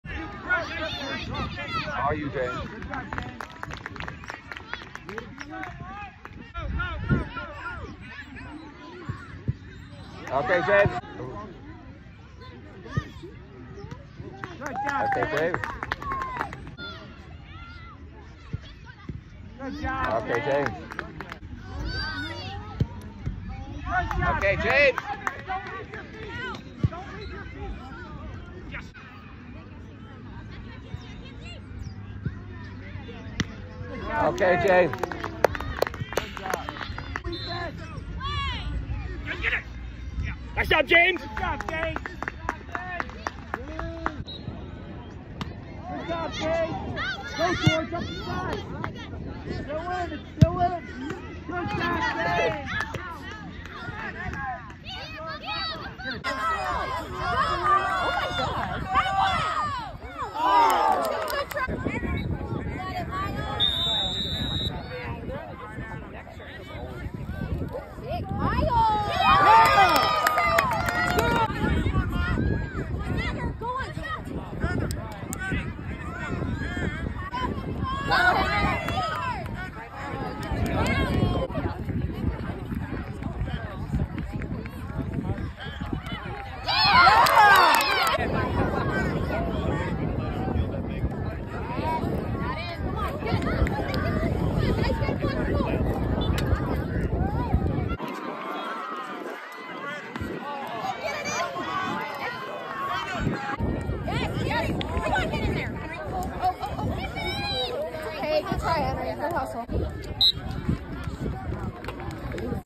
James' work in his 6-2 win vs Sporting Blue Valley Academy Navy 15. Game 2.
Orland Park Sting FC 2015 Premier: 6
Sporting Blue Valley Academy 2015 Navy: 2
2024 SLSG Fall Fest
October 12, 2024
O'Fallon, IL